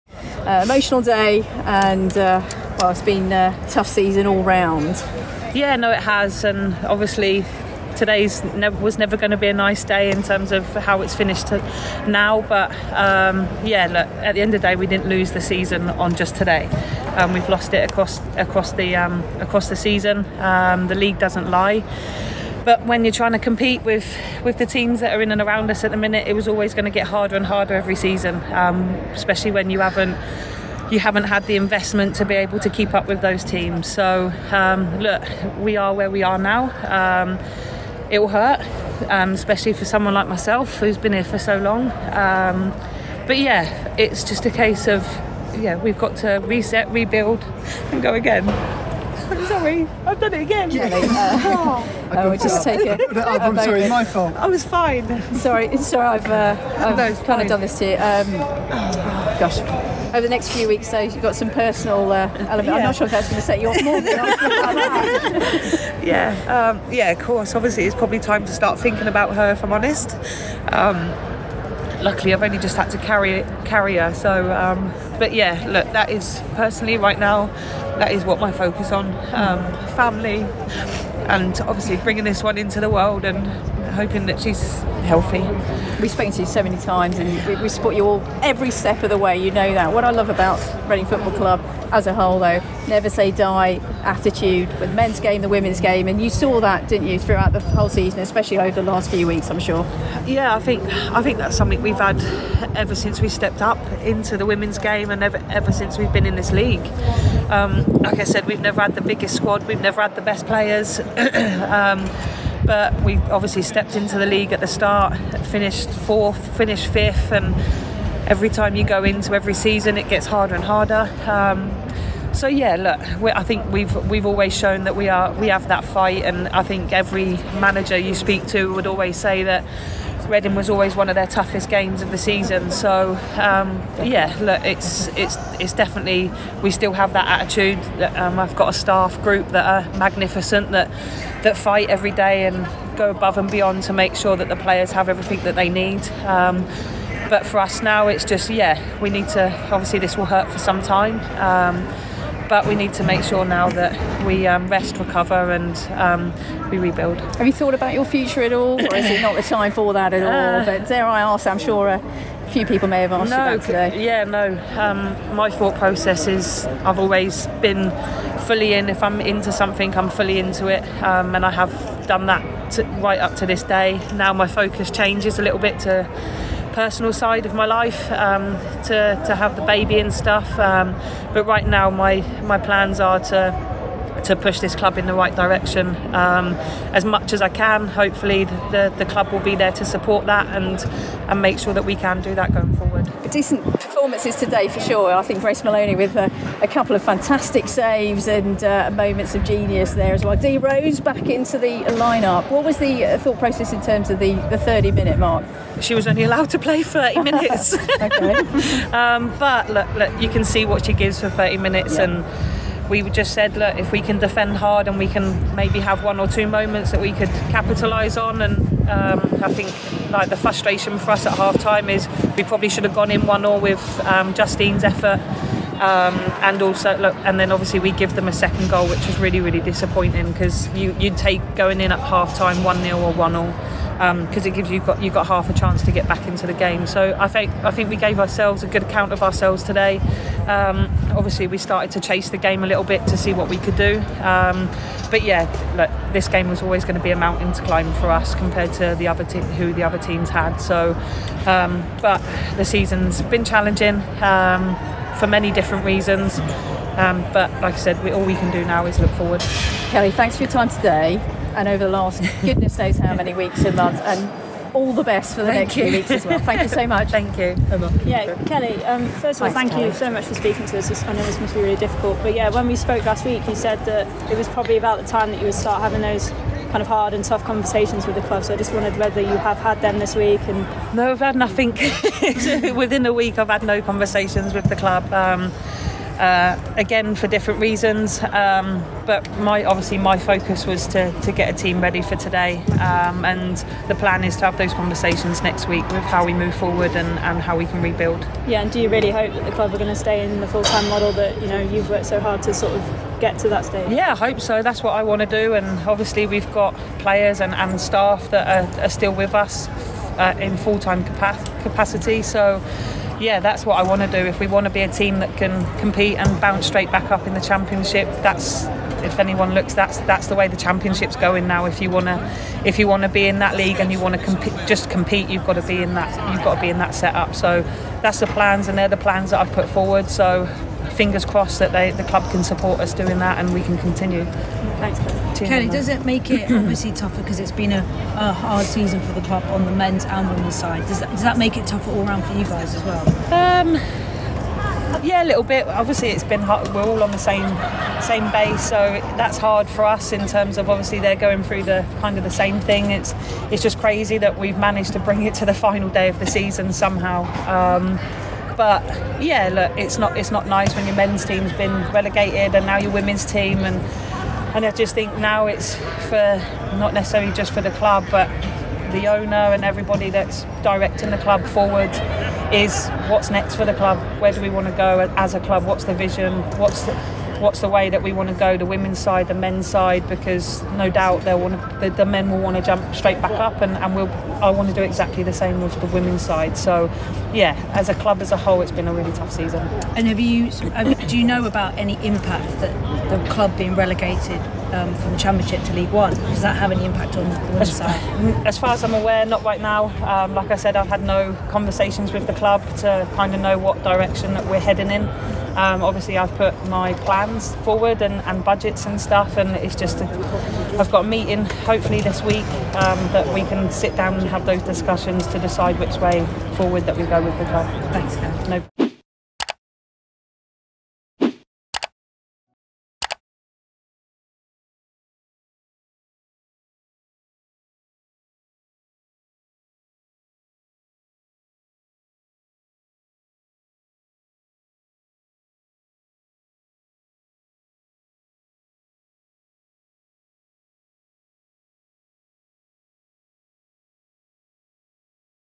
Kelly Chambers FULL post-match press conference | Reading 0-3 Chelsea | WSL
Reading boss Kelly Chambers holds a post-match press conference following Reading’s 0-3 loss to Chelsea.